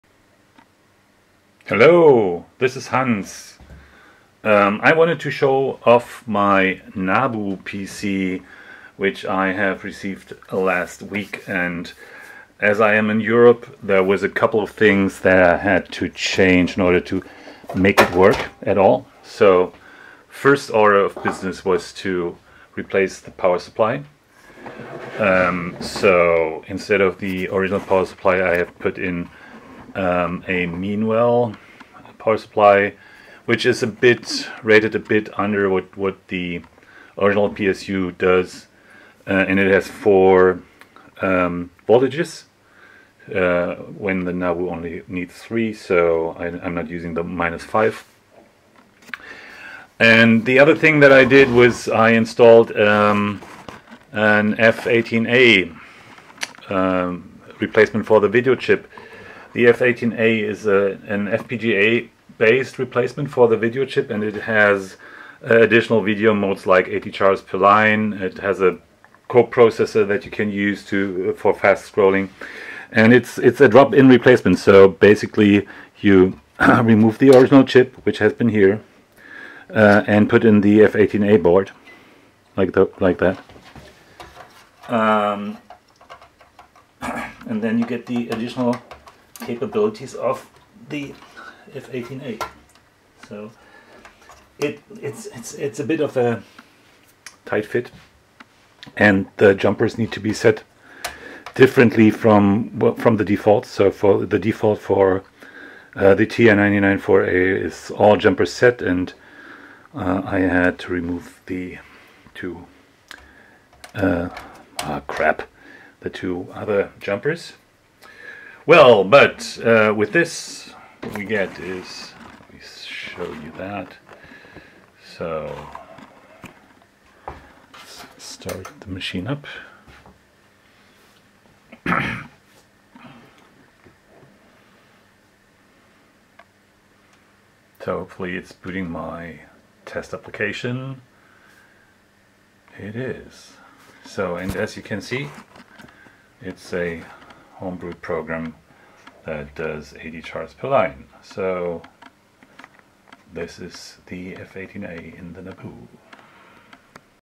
My NABU has a 230V PSU and the F18A VDP replacement
I installed a Mean Well Q-60B power supply.  The NABU only needs +5V, +12V and -12V, so the -5V rail remains unused.

STL file for the replacement panel to make the VGA output accessible

I forgot to mention that the F18A outputs VGA, so there :)